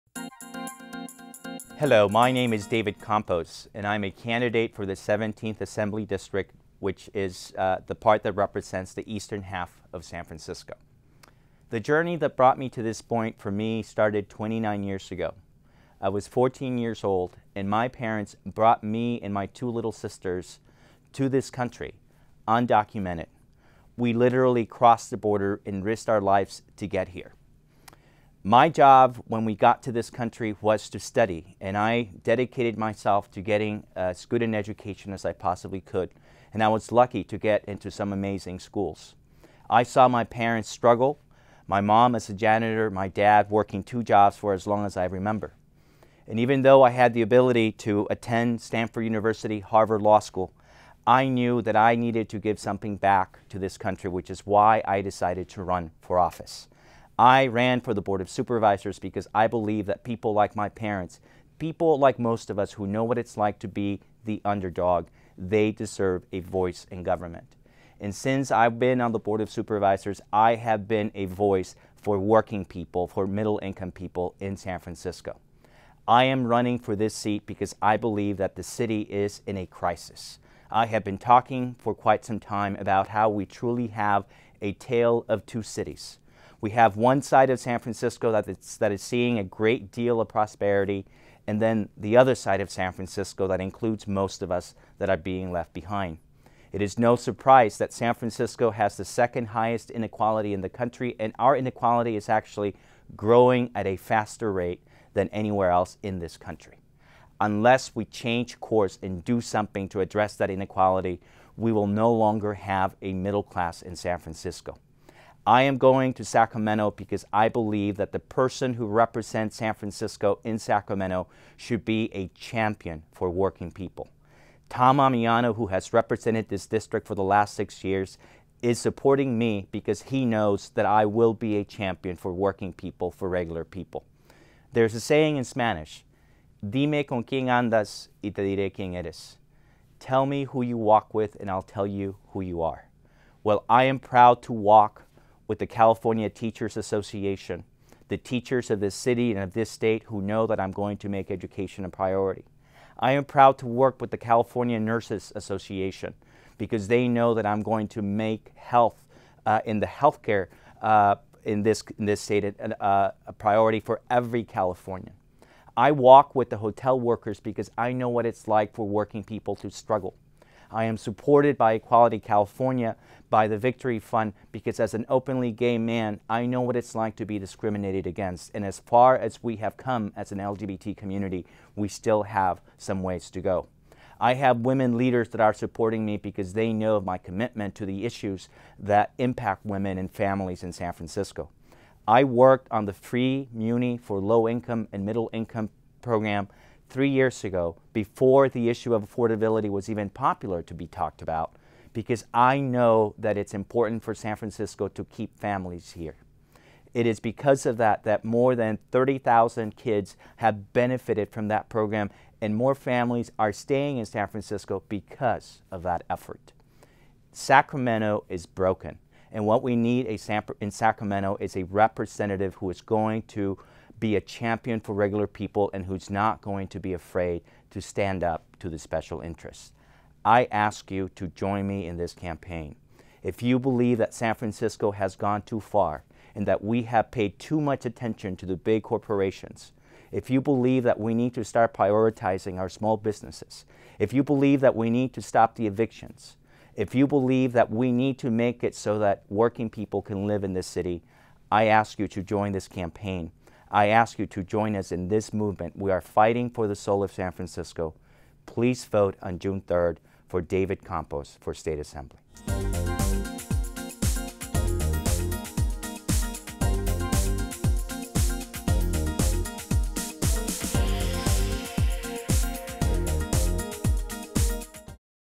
David Campos - Candidate for State Assembly, District 17
David Campos, Candidate for State Assembly, District 17, speaks about his candidacy, and important issues facing San Francisco.